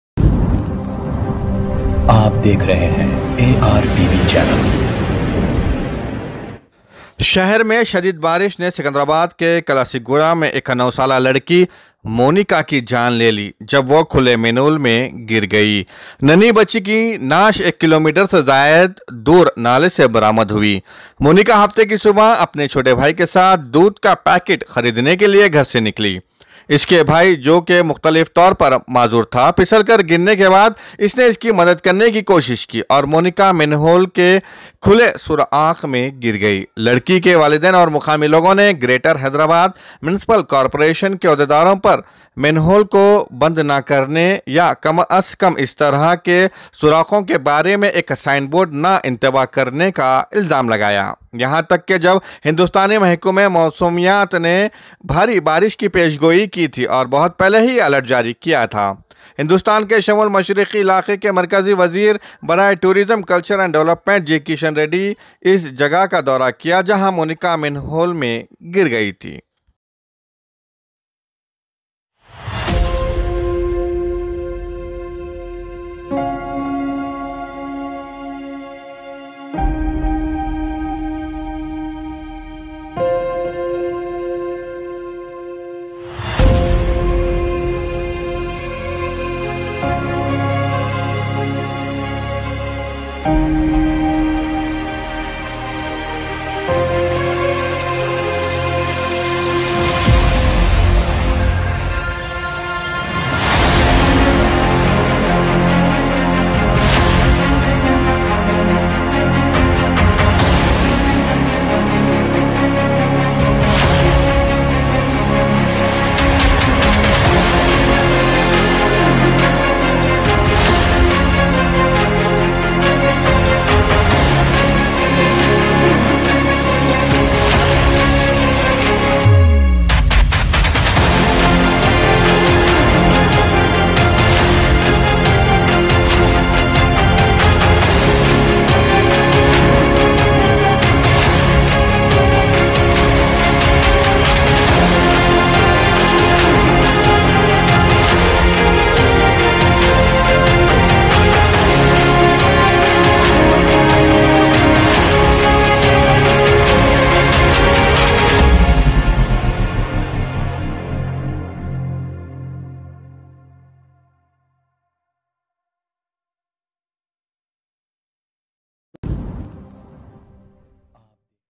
09 years old girl Mounika Drowns in open manhole
09 years old girl Mounika Drowns in open